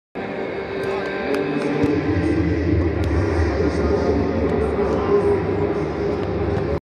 Wilmar Barrios, a los golpes con un colega en Zenit
En la previa al duelo frente al Chelsea, Wilmar Barrios fue encontrado a los golpes dentro del Zenit de San Petersburgo con un compañero. ¿Qué sucedió?